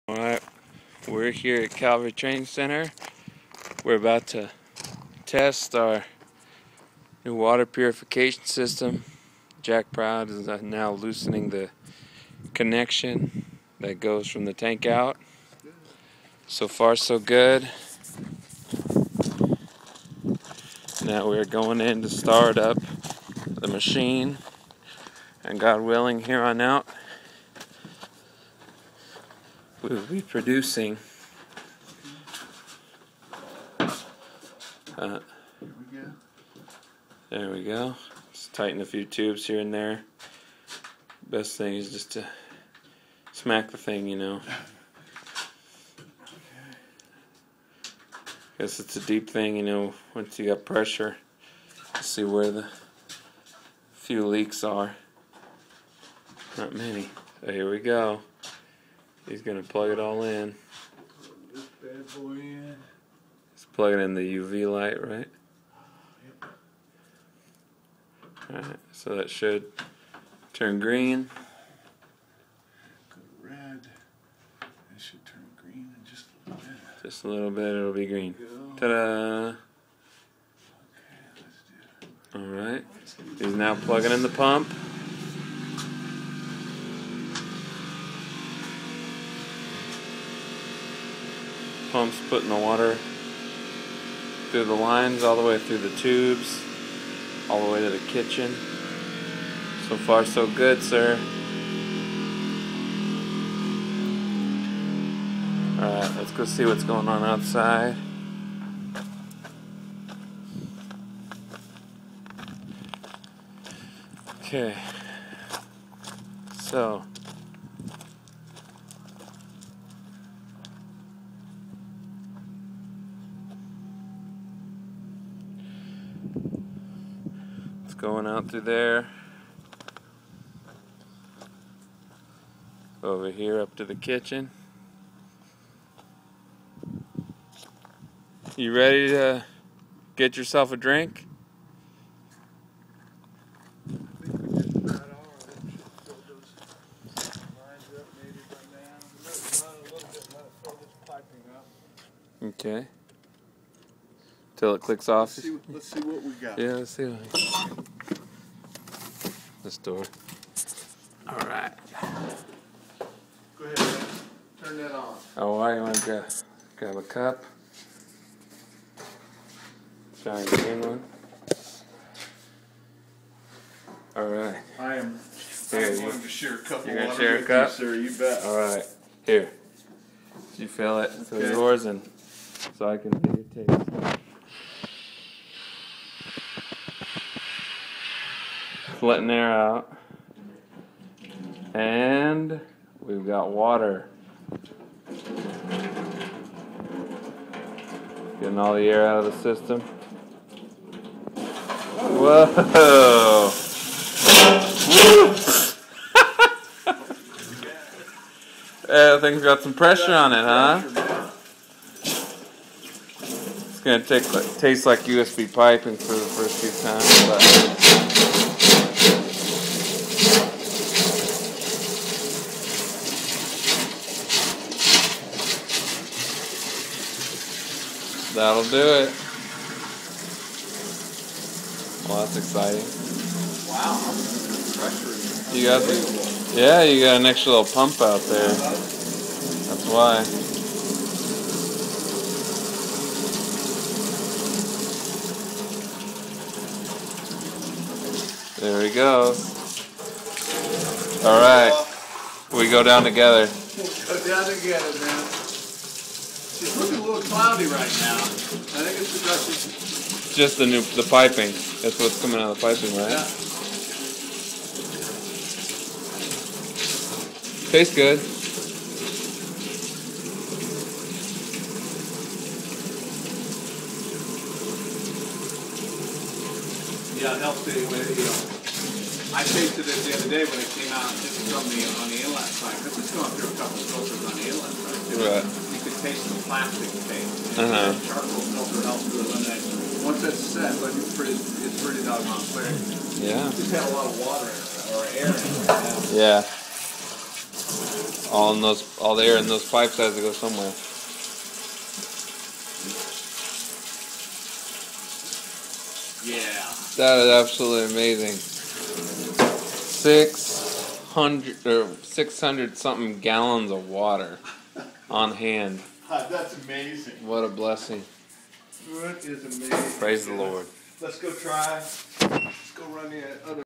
Calvary Training Center Clean Water Ministry Now in Full Swing!
Calvary Training Center is a missionary facility in La Gloria, Tijuana, Mexico. We were donated a $5000+ reverse osmosis water purification system. Our plan is to sell water to those who can afford it to bless the ministry and donate water to those who cannot afford clean water.